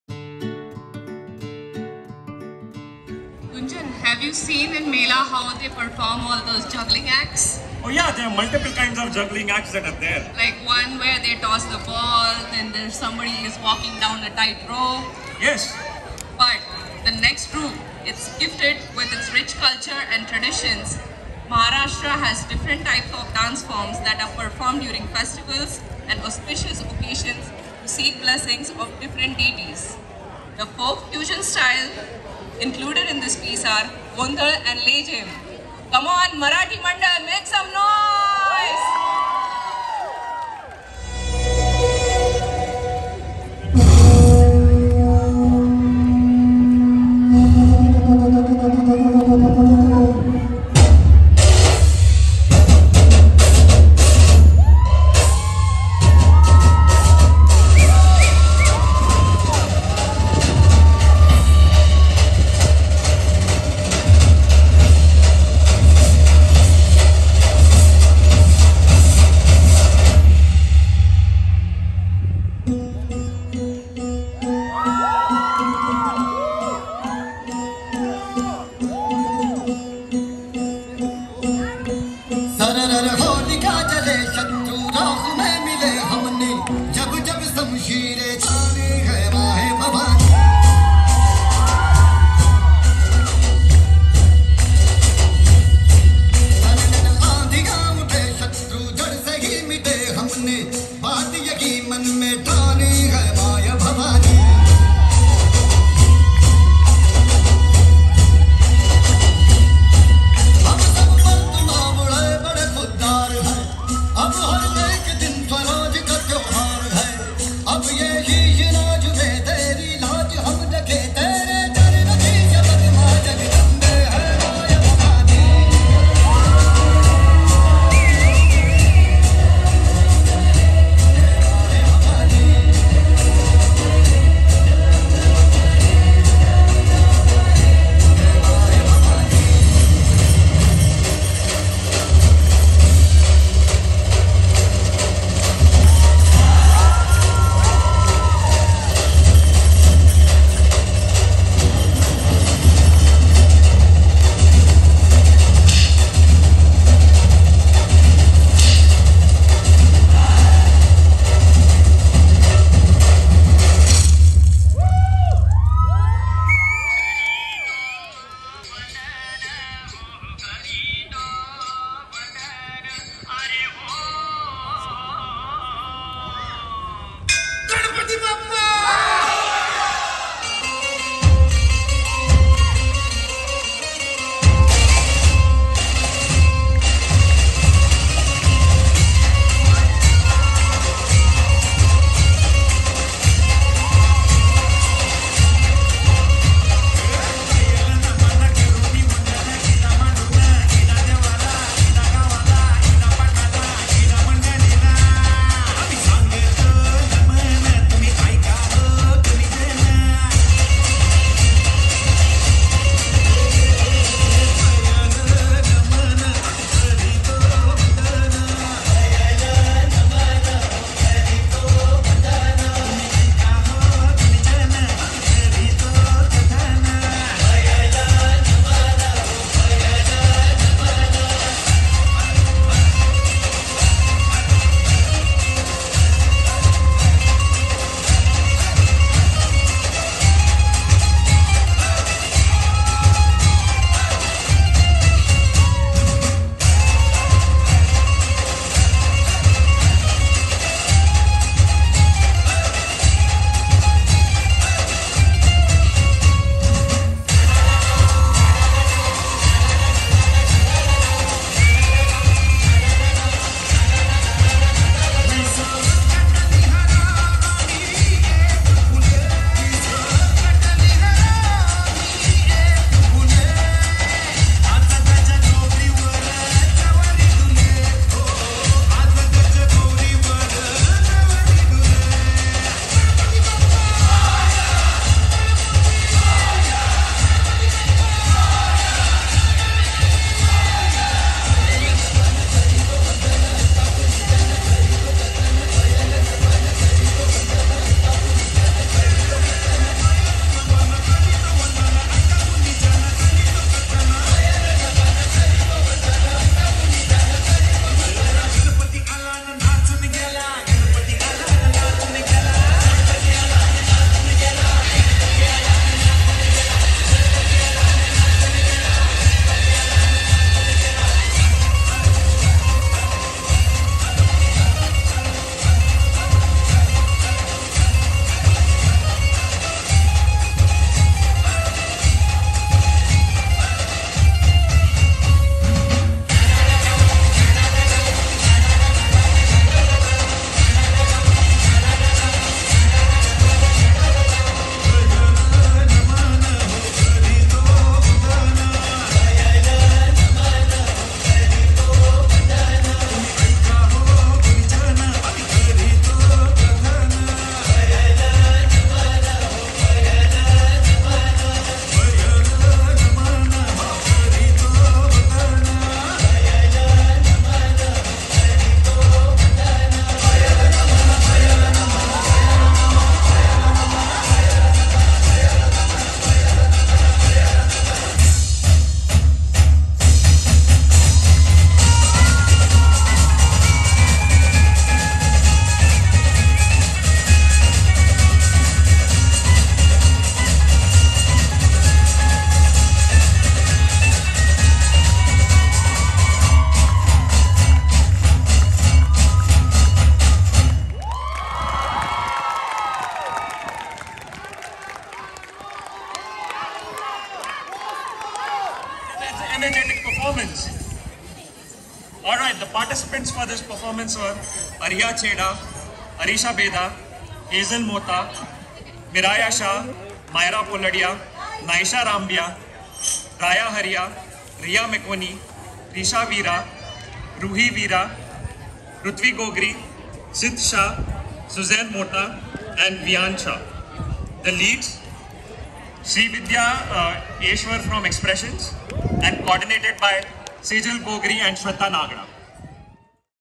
Aamhi Marathi - KV Carnival 2022 Performance
Performance at KutchValley's Diwali celebration event - KV Carnival 2022

Participants : Ariha Chheda, Arisha Bheda, Hazel Mota, Miraya Shah, Myra Poladia, Nysha Rambhia, Raya Haria, Ria Meckoni, Risha Veera, Ruhi Veera, Rutvi Gogri, Siddh Shah, Suzanne Mota, Viaan Shah
Parent Lead : Sejal Gogri, Sweta Nagda